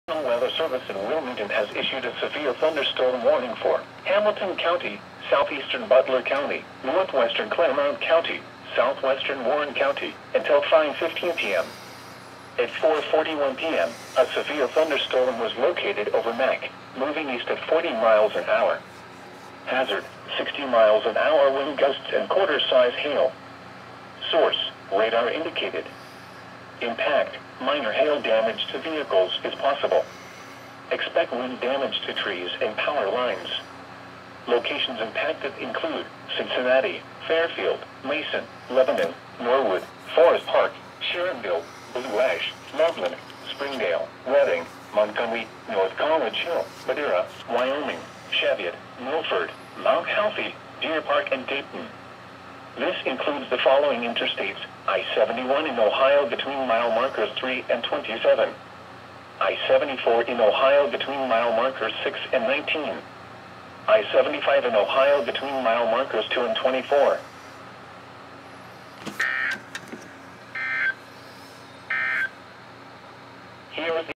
Severe Thunderstorm Warning #4 (EAS #462)
Final alert for 5/10/20.